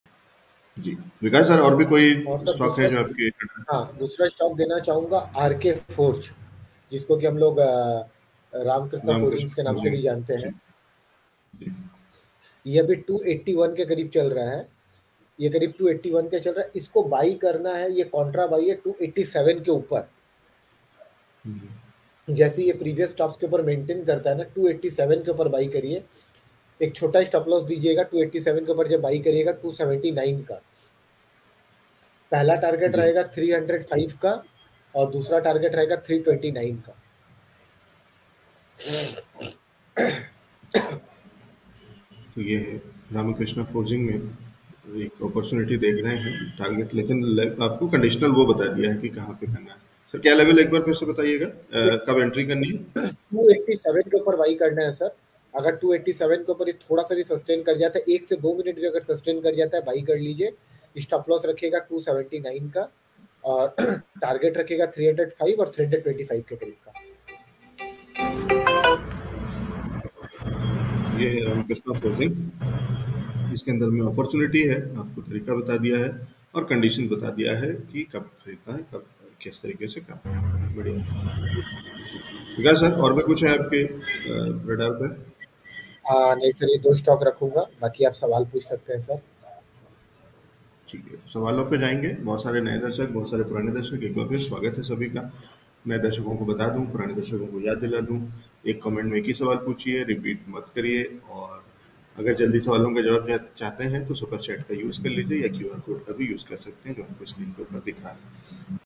Ramkrishna Forgings Ltd. के शेयर में क्या करें? Expert Opinion by Vikash Bagaria
जानिए Stock Market Expert  Vikash Bagaria की राय |

Analysis of: Ramkrishna Forgings Ltd.

Ramkrishna Forgings Ltd.
Shows on StockMarket equity trading investment commodity personalfinance reviews marketanalysis startups nifty sensex and much more.